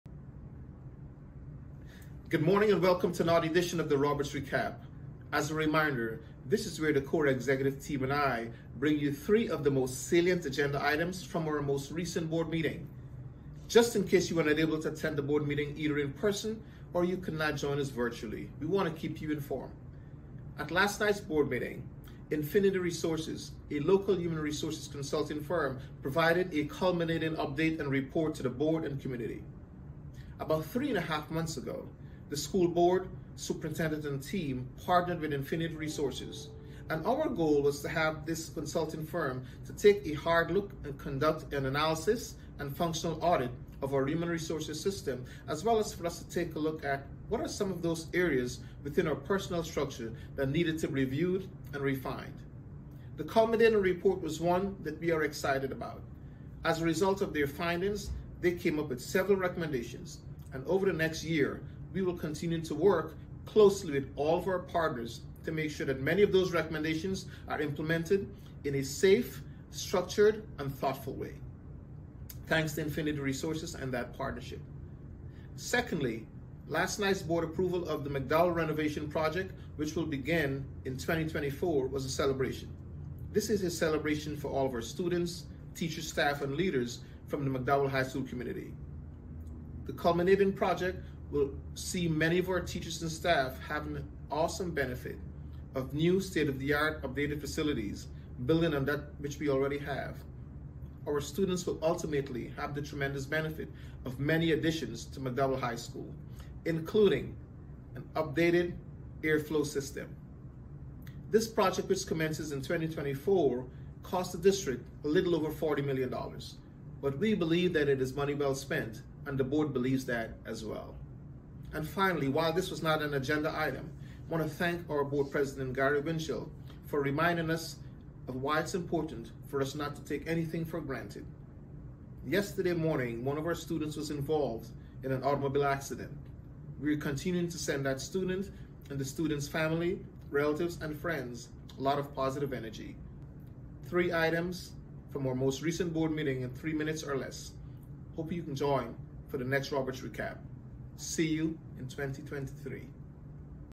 Roberts Recap 12/12/22
In case you missed last night's School Board meeting, here is the Roberts Recap to give you the highlights in 3 minutes or less!